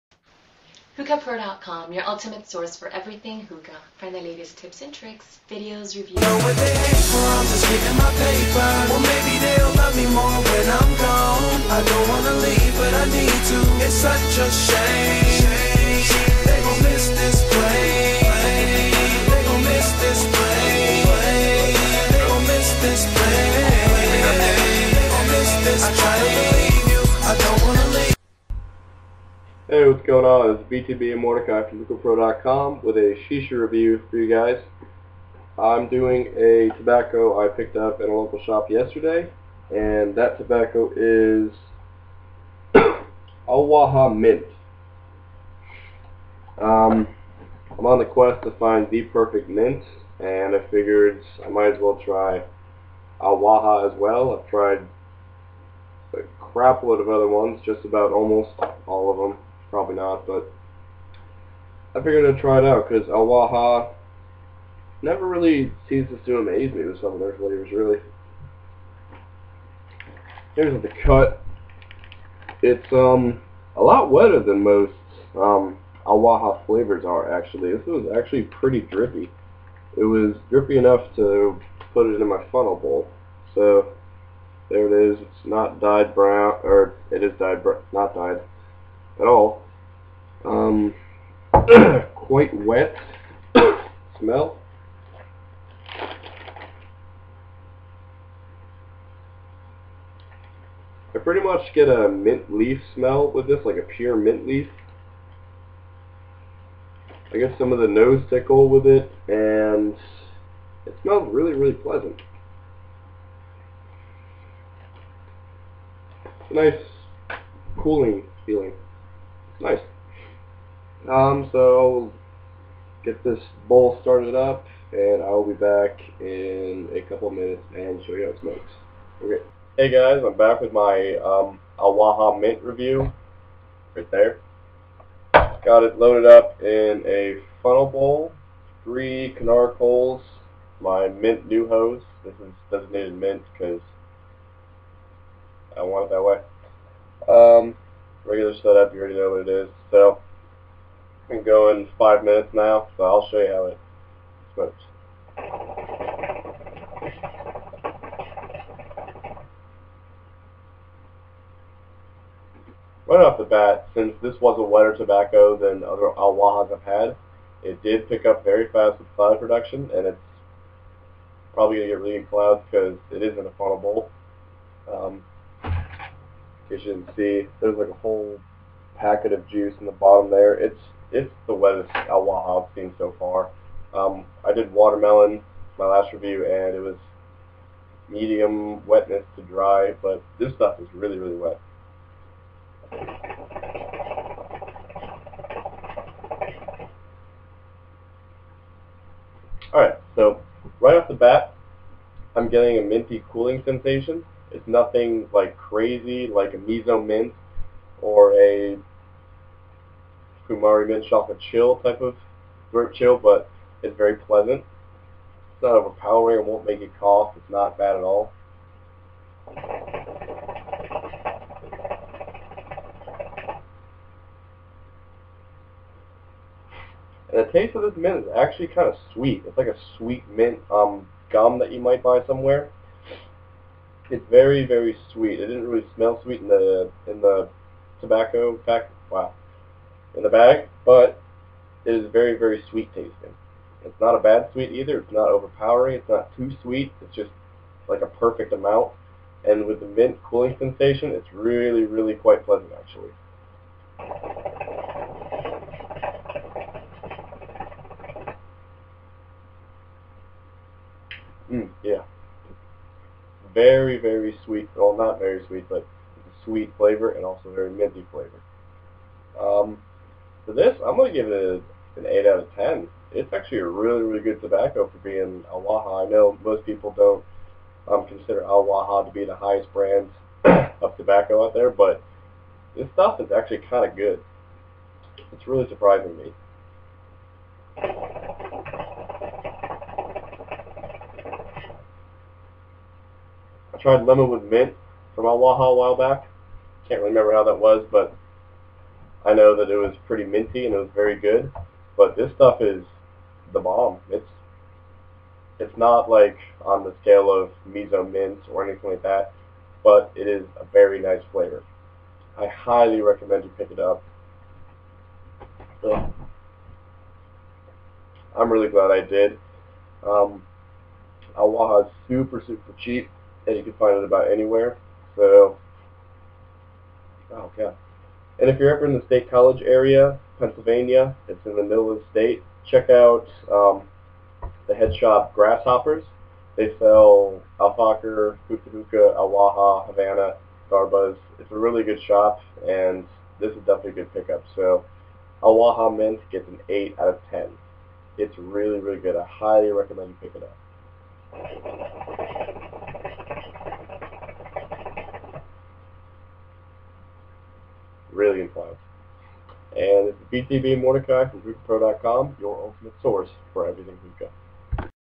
Al Waha Mint
Highly recomend this flavor. Great mint taste